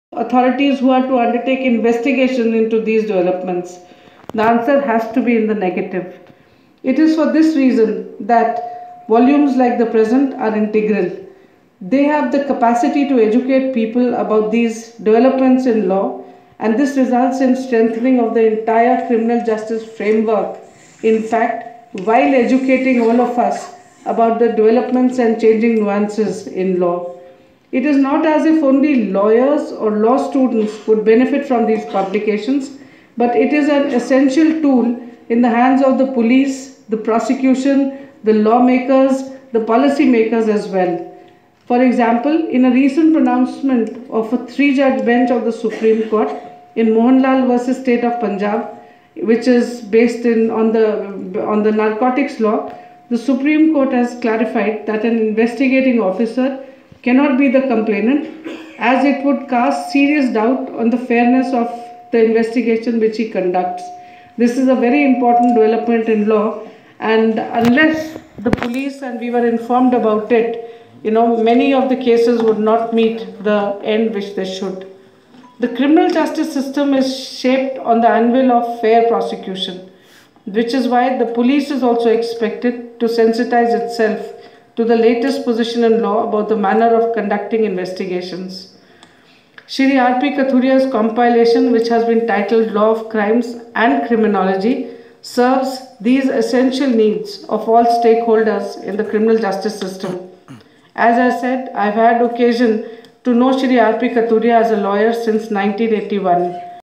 Geeta Mittal, Chief Justice presides as chief guest at release of R.P. Kathuria's
Geeta Mittal, Chief Justice presides as chief guest at release of R.P. Kathuria's "Law of Crimes and Criminology"